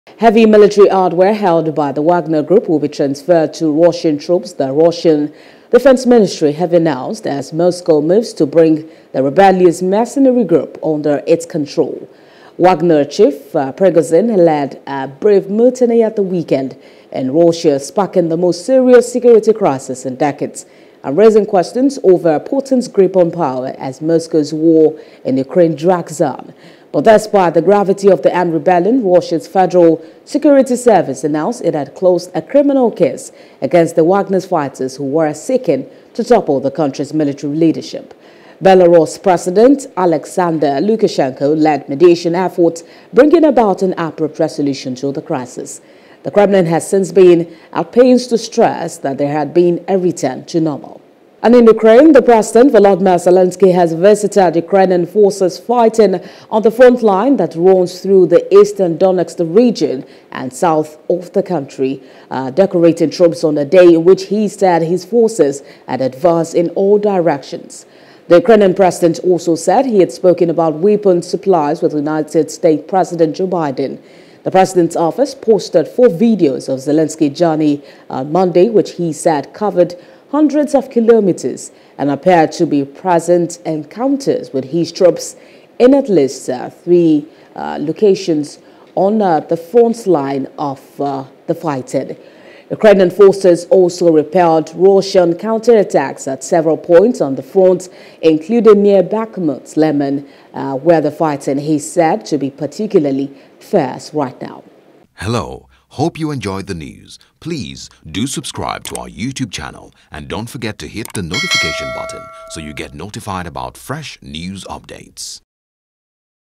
Ukraine: Zelenskyy Says Frontline Forces Are Advancing In All Directions
Ukrainian President Volodymyr Zelenskyy has visited Ukrainian forces fighting on the front line that runs through the eastern Donetsk region and the south of the country, decorating troops on a day in which he said his forces had “advanced in all directions”.